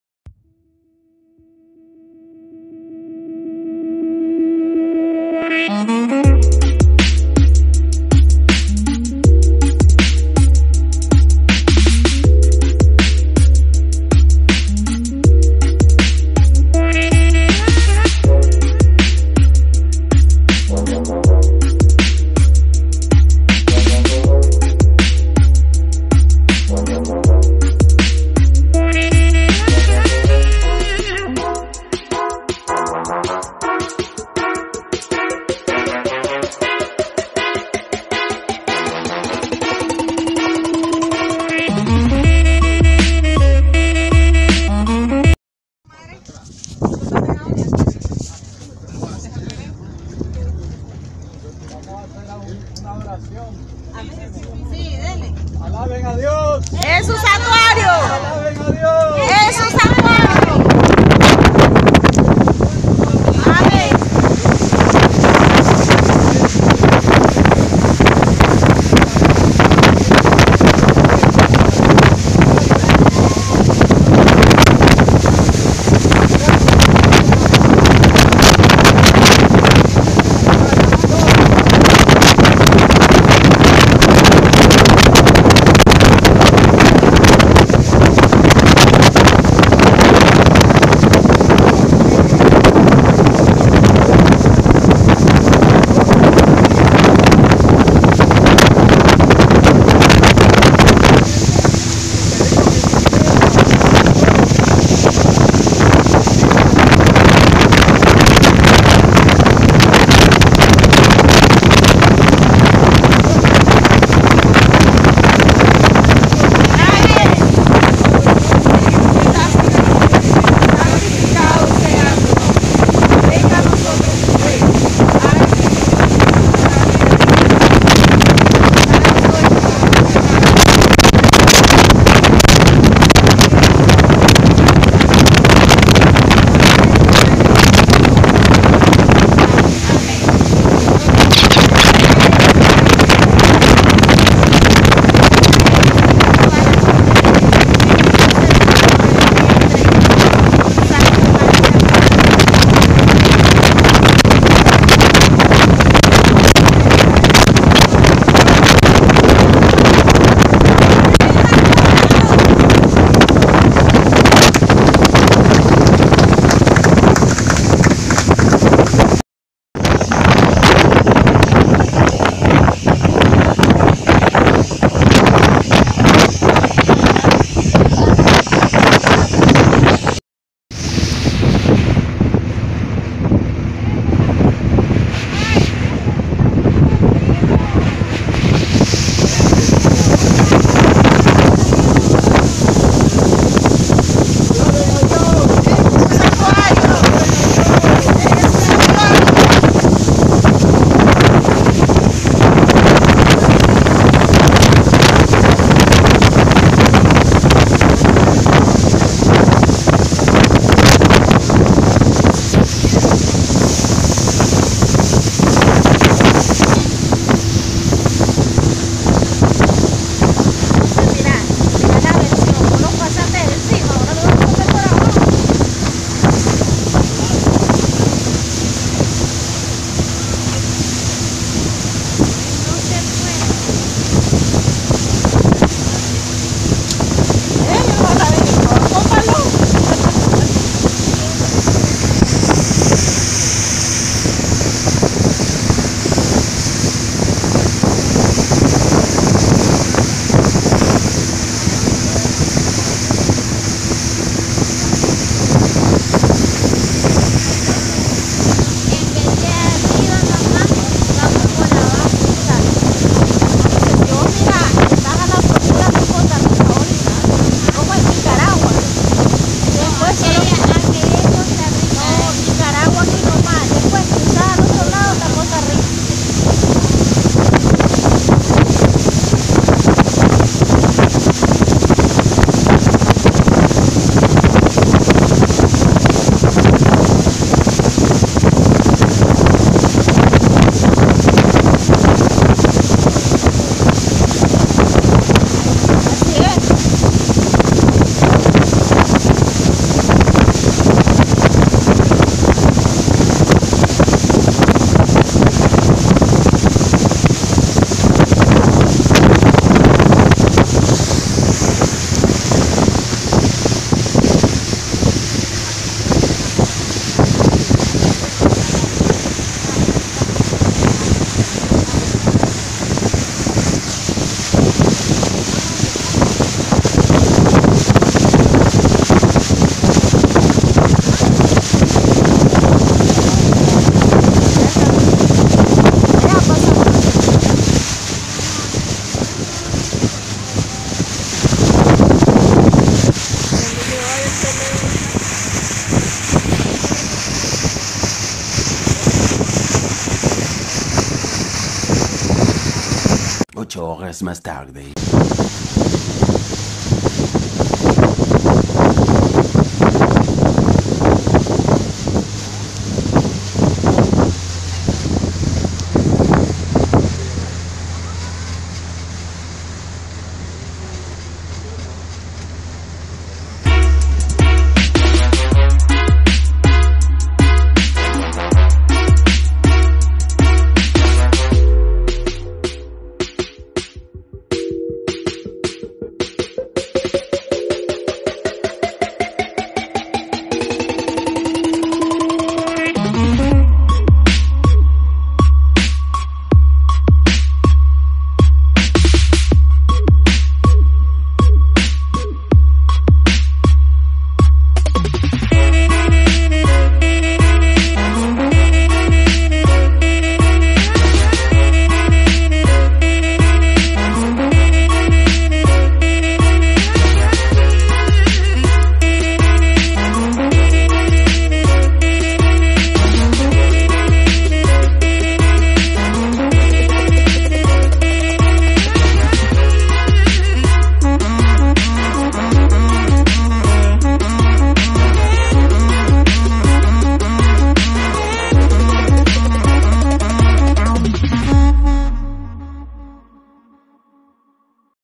EL CASTILLO NICARAGUA 2018
SÁBADO 10 DE MARZO 2018

Experiencia super linda les comparto este pequeño vlog espero les guste, pasamos 2 días en el castillo, fue uno de los mejores viajes en ese año, no grabé mucho porque todavía no entraba al mundo de los creadores de contenido, pero esto fue un poco de lo que pude recuperar de ese viaje, les cuento avía más vídeos de una excursión por el museo, una historia muy bonita de mi país Nicaragua pero bueno eso me quedo solo en recuerdos.